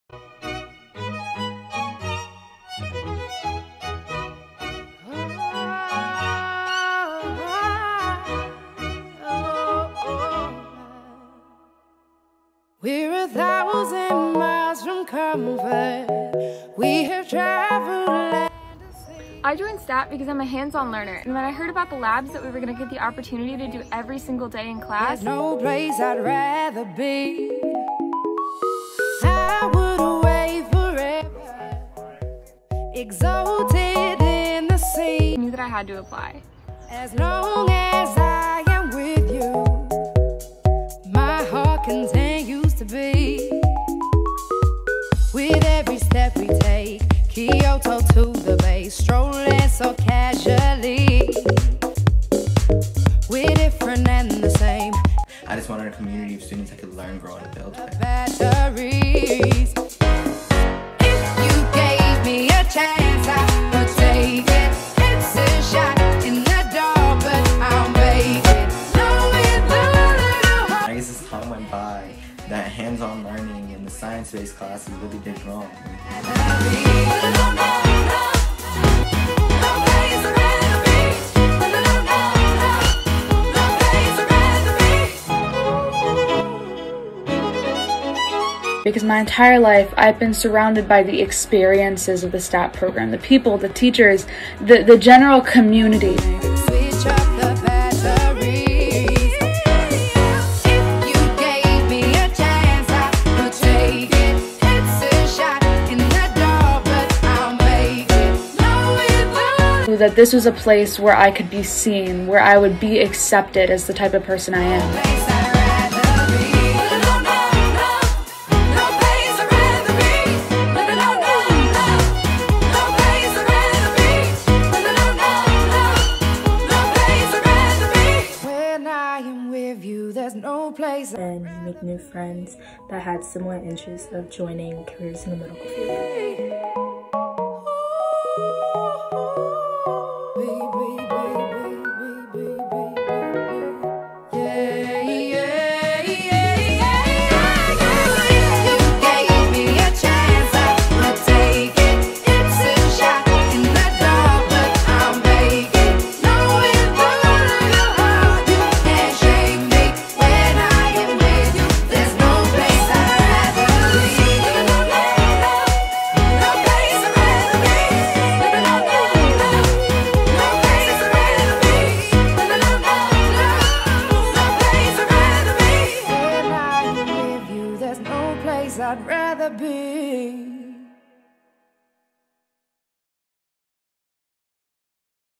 Career & College Pathways Night: STAT Biomed
Do you want to learn more about Stafford County Public Schools' Stafford Academy for Technology (STAT) Biomed Program? Watch this video as part of the 2020 Virtual Career & College Pathways Night.

Video was previously recorded.
Created by: Esme A. and Emma R.